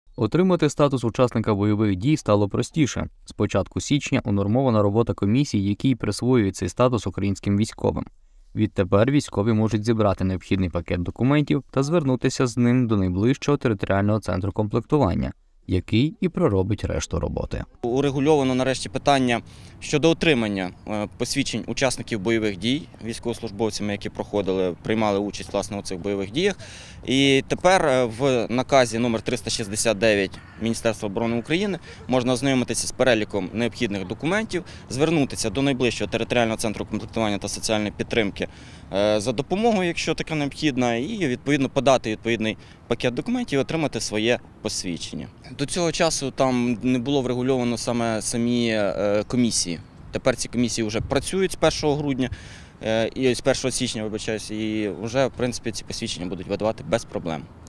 Отримати статус учасника бойових дій стало простіше
Отримати статут учасника бойових дій стало простіше. З початку січня унормована робота комісій, які й присвоюють цей статус українським військовим.

Відтепер військові можуть зібрати необхідний пакет документів та звернутися з ним до найближчого територіального центру комплектування, який й проробить решту роботи.